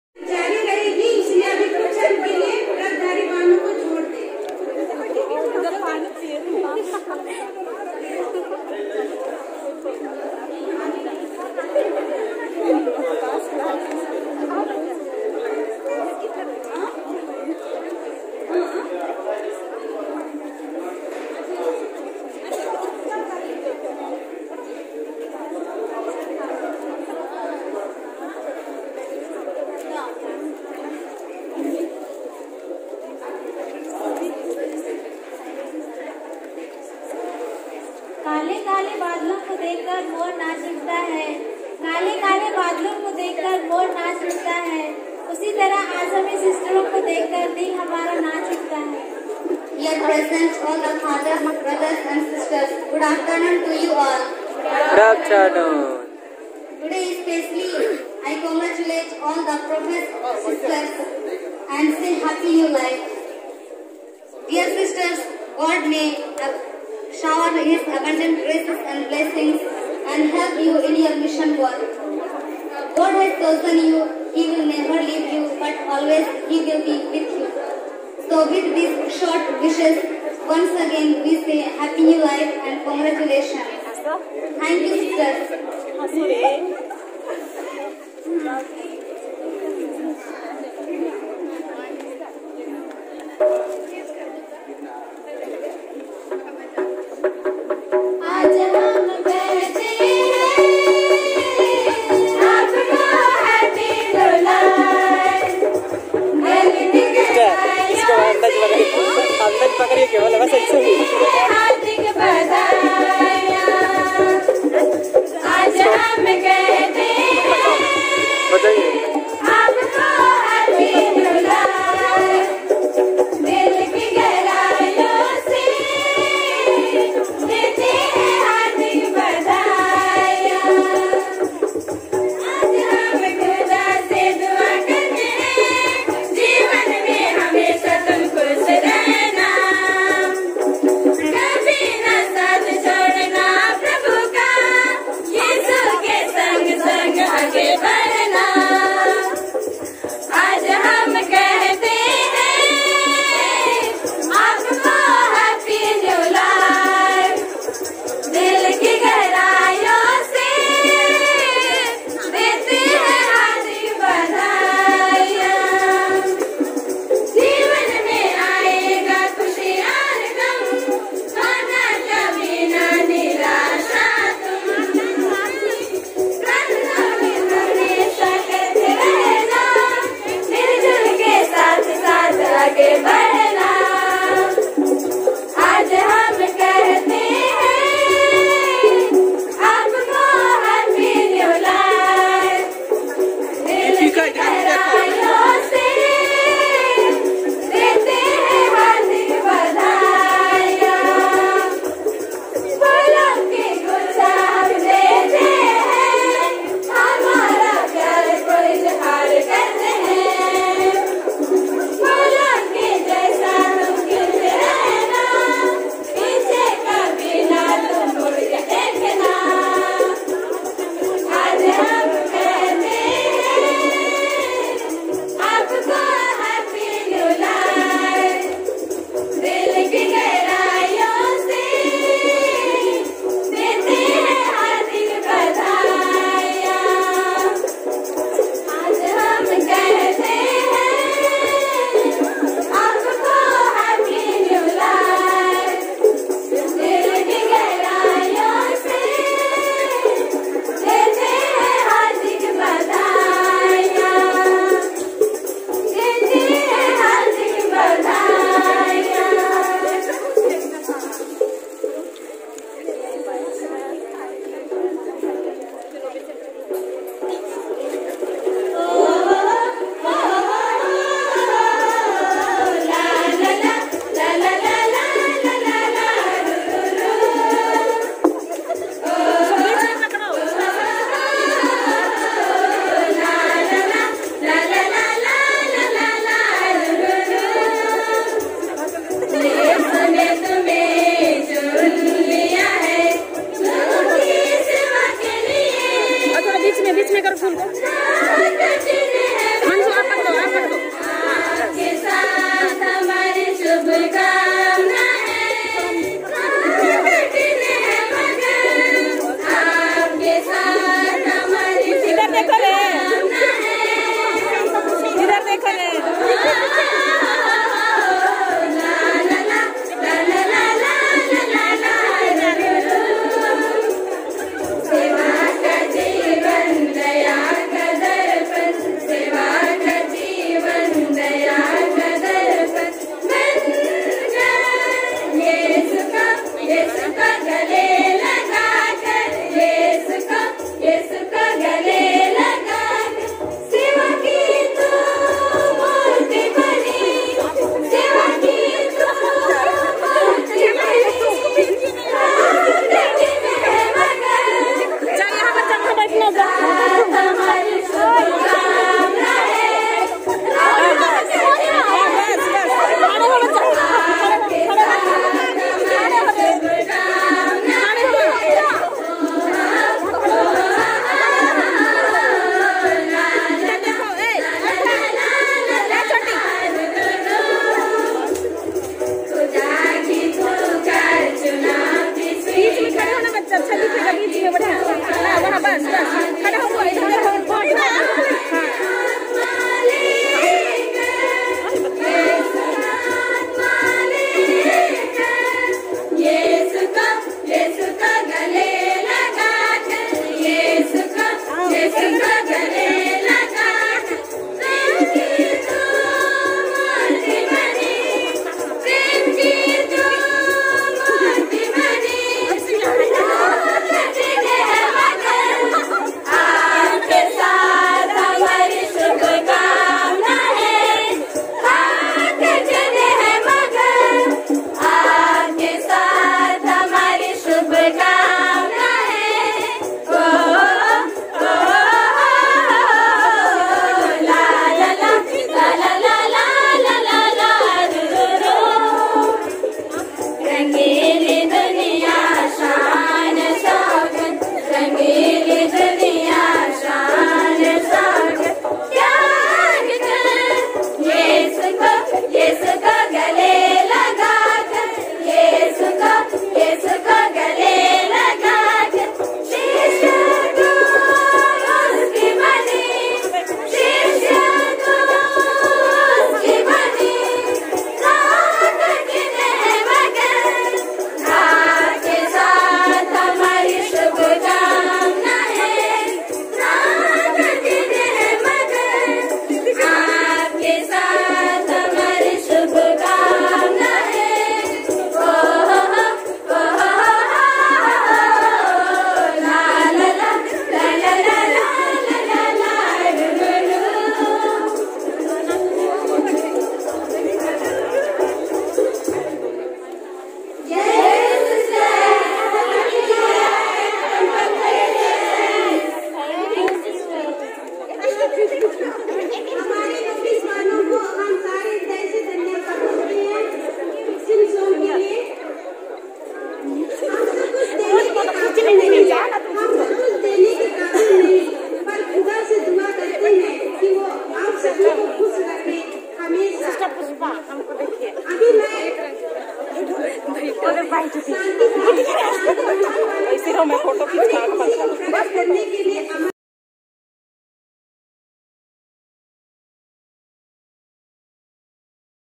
Final profession kunkuri 2020🙏🙏
Today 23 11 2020 Final profession is taken by 6 St.Anne sisters. May God Shower his blessings upon them.And may fill in Thier life with his love Joy and happiness.so that they may proclaim the word of God to all the people.

Congratulations dear sisters.
Happy new life dear sisters.